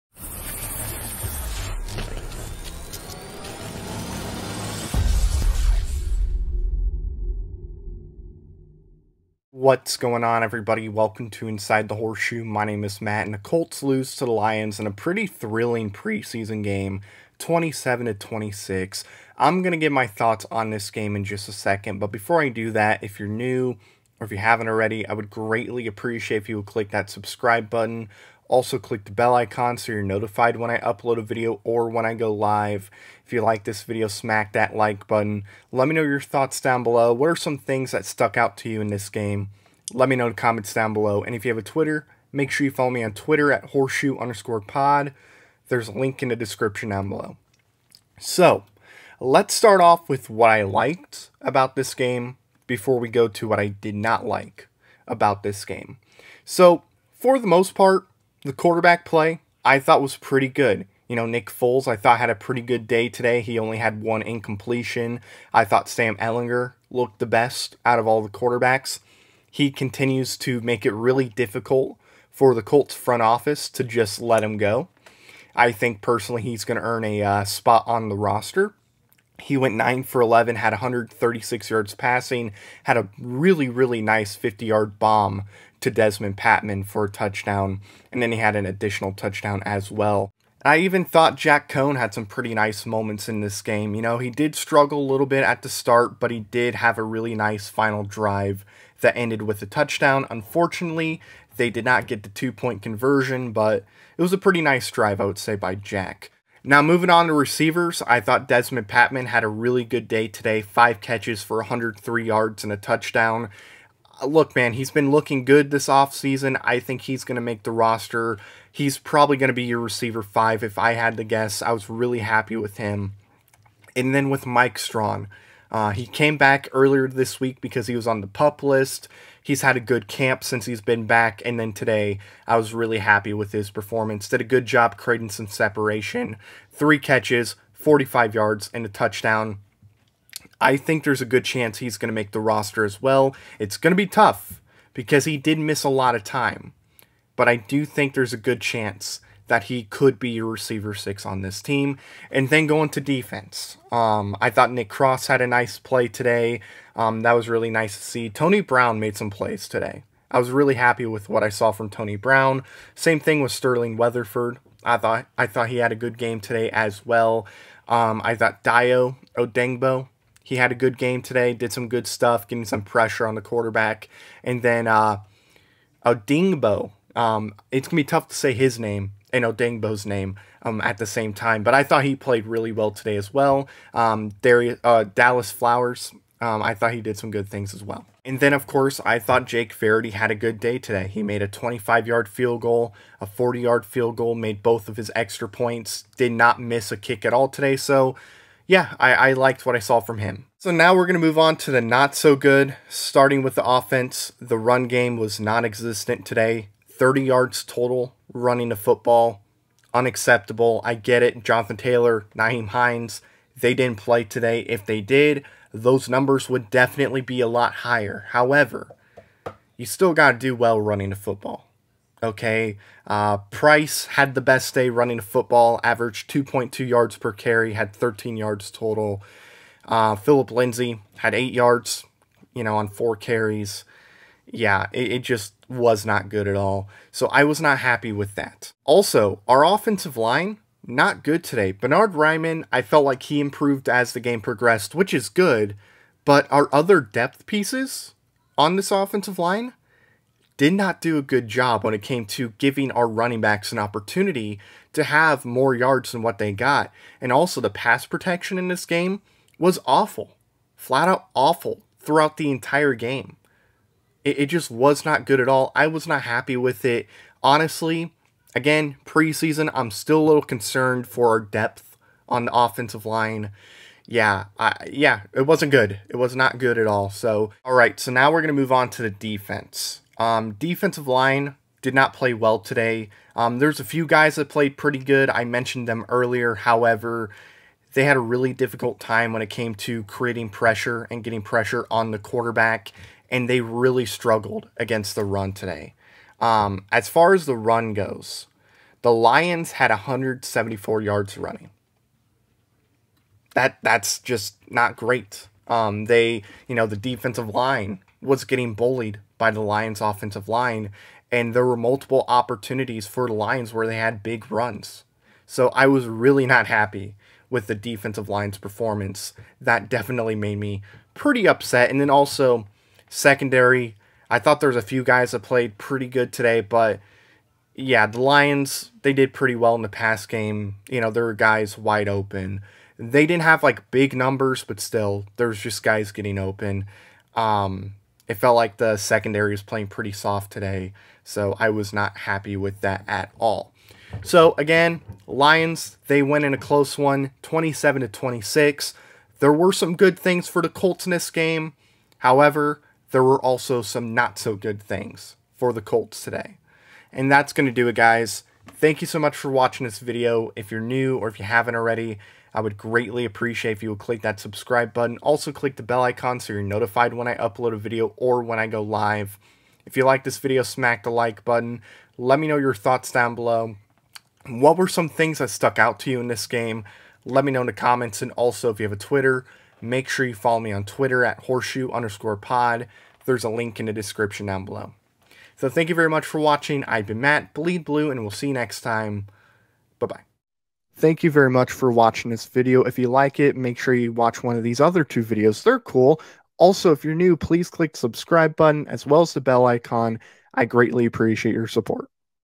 Colts Lose To Lions in 2nd Preseason Game
The Colts unfortunately lose to the Lions 27 to 26 in their 2nd preseason game. They are 0-2 in the Preseason this year